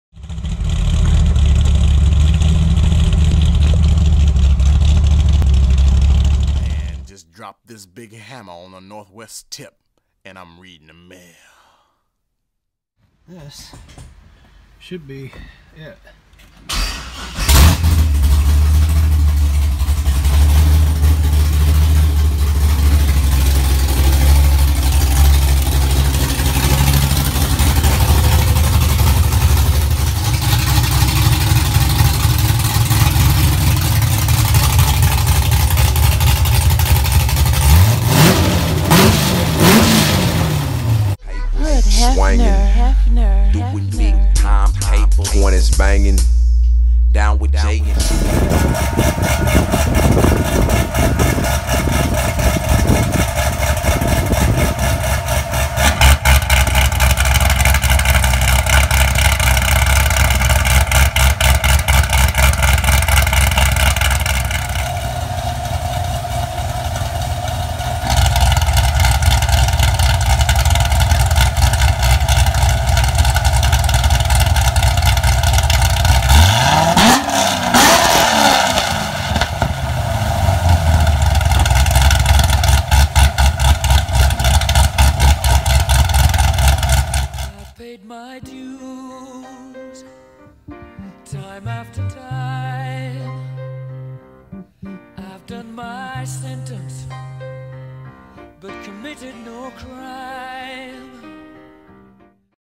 2018 Mods are on
C6 Z06 got some upgrades this year! Check out the video for all the new goods.